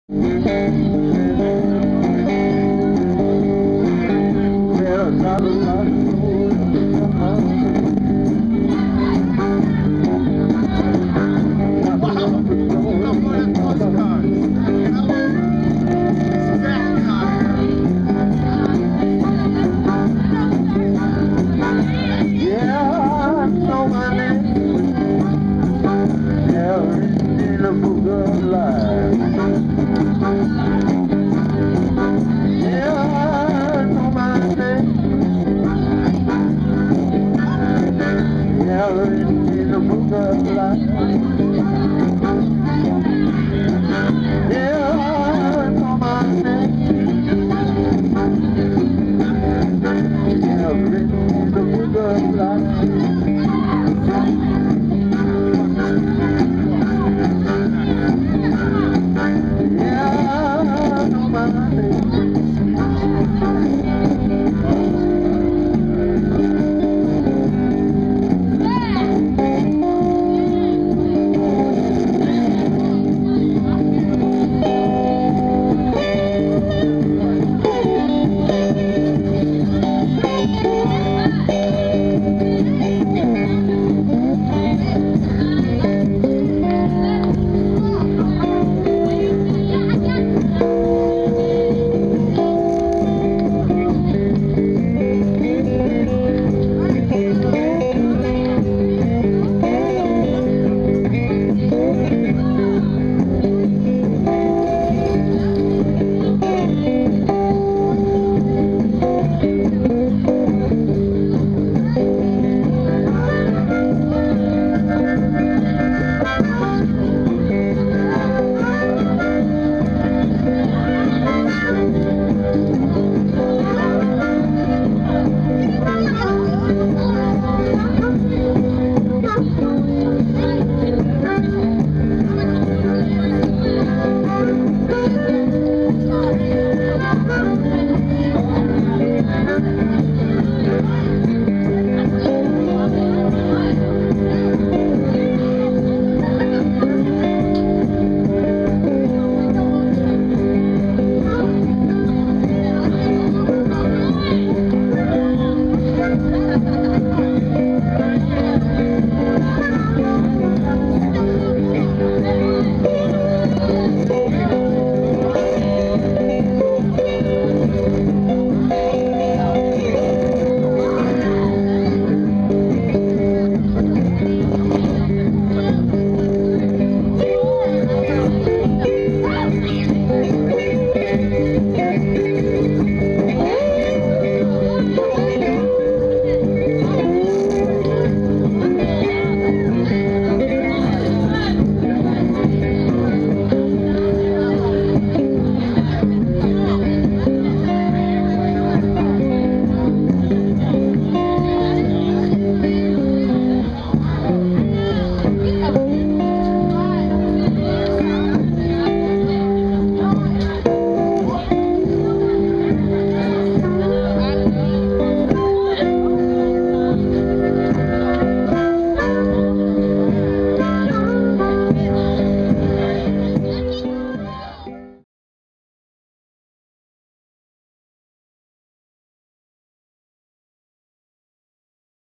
Stoker @ The King Biscuit Blues Festival, Helena, Ark. - Oct. 9, 2010
Stoker @ The 25th Annual King Biscuit Blues Festival, Helena, Ark. - Oct. 9, 2010